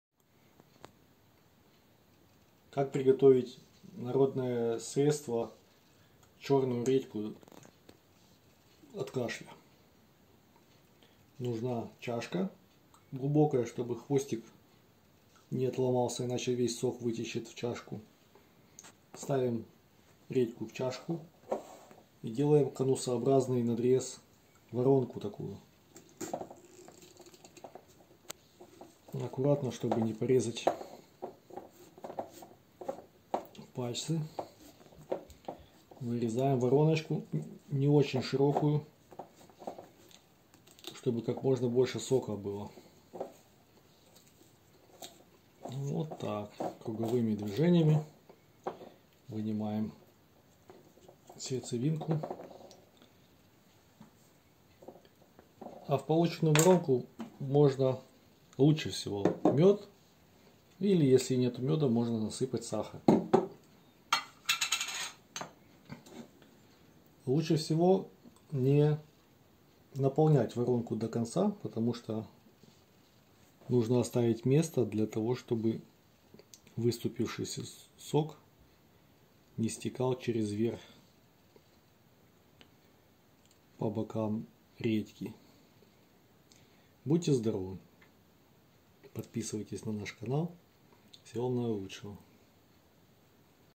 Черная редька от кашля. Как готовить, какую посуду лучше выбрать? Натуральное вкусное лекарство.
Давно принимаю черную редьку от кашля, с самого детства. Родители ещё готовили такой сироп. Папа выращивал на своем огороде, хранил в погребе и по мере необходимости доставал и мы делали из неё "Горшочек". Внутрь наливали натуральный мёд, который потом, смешиваясь с соком редьки, превращался в лекарство.
Помогает от кашля хорошо. Знаю, что беременным нельзя, а так безопасно. Рекомендую!.